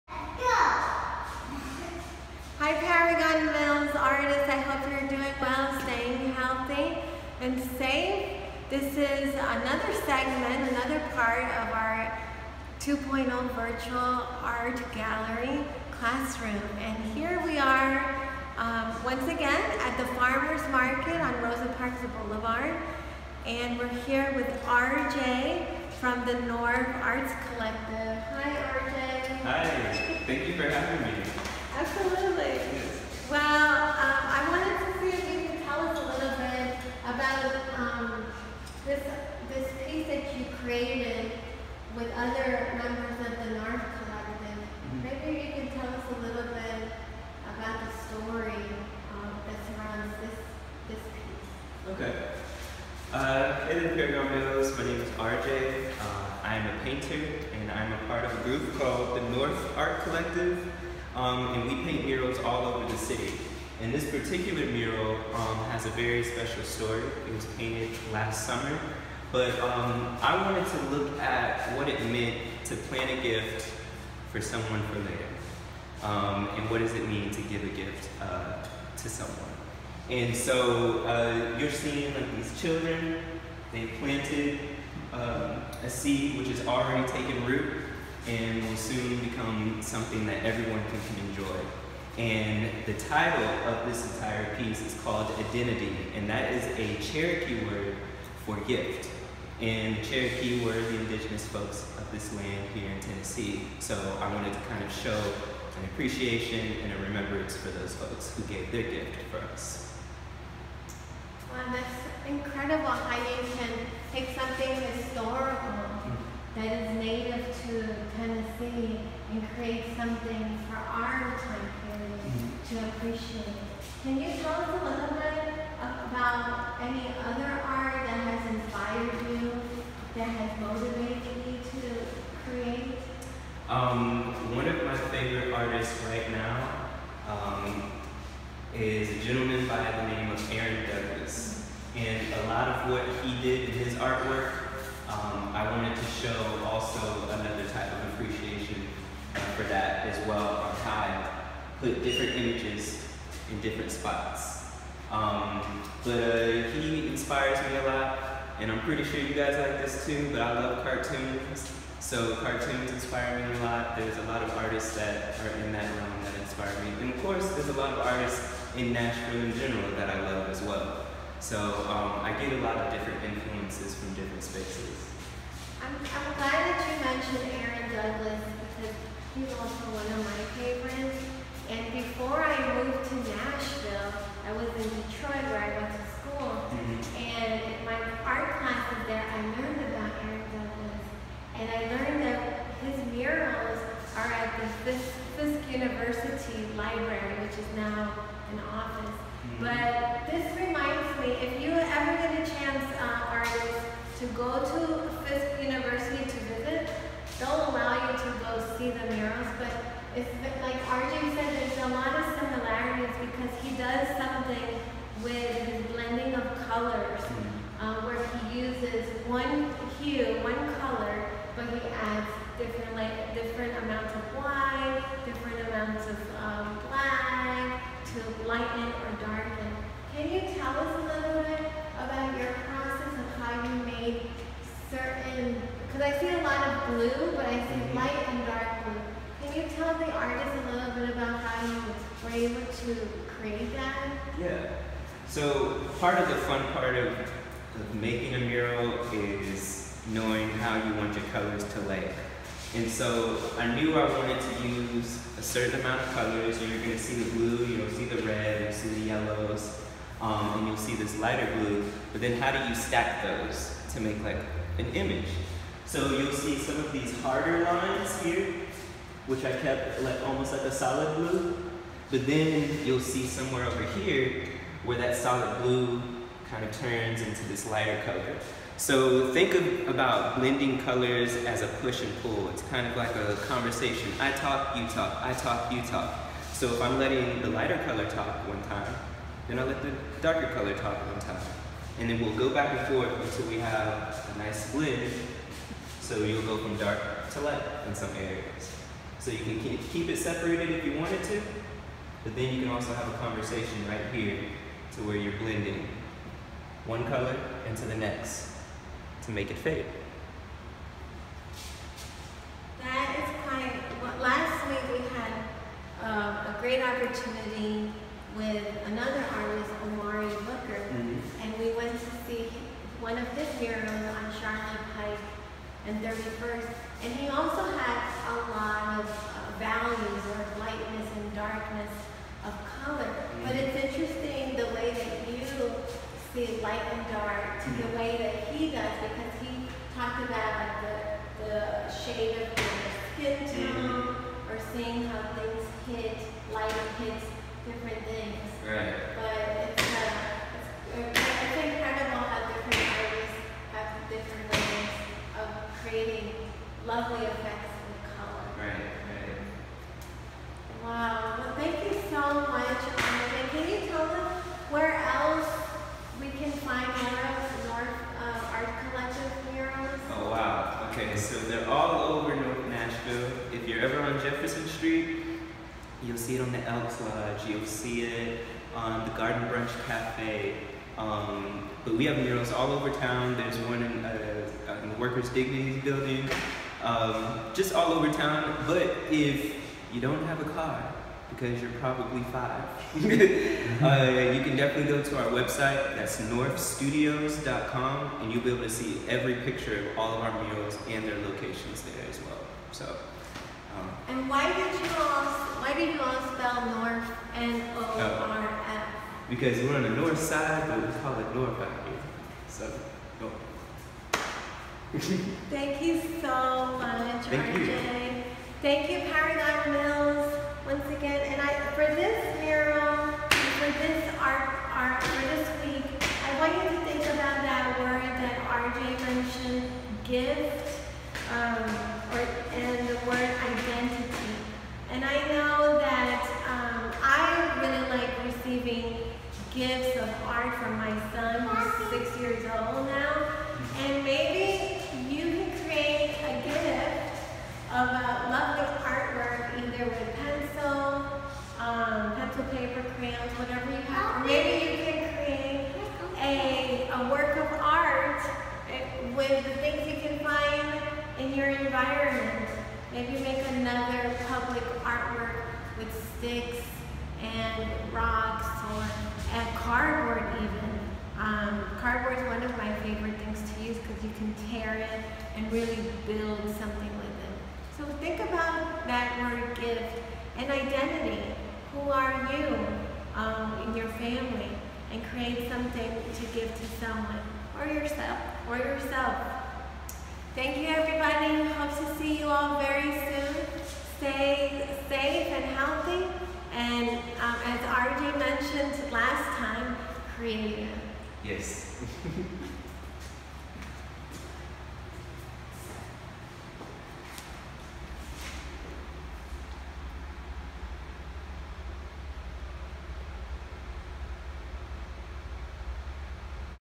Norf Art Collective with Arjae Thompson ~ Mrs. Negrón Paragon Mills Elementary Art Teacher
Mrs. Negrón took the students to a (virtual) art tour of Norf Art Collective's mural located at the Nashville Farmer's Market on Rosa Parks Blvd. There we met with one of the muralist of the collective, Arjae Thompson who spoke to us about the roots of the mural he created and how it was dedicated to the Cherokee Tribe, native to TN and who lived on the same lands the farmer's market stands on today. The mural can be viewed on the new entryway vestibule. The mural is titled: Adanedi. "Named for the Cherokee word for “gift,” the two murals feature beautiful interpretations of planting, growth and harvesting."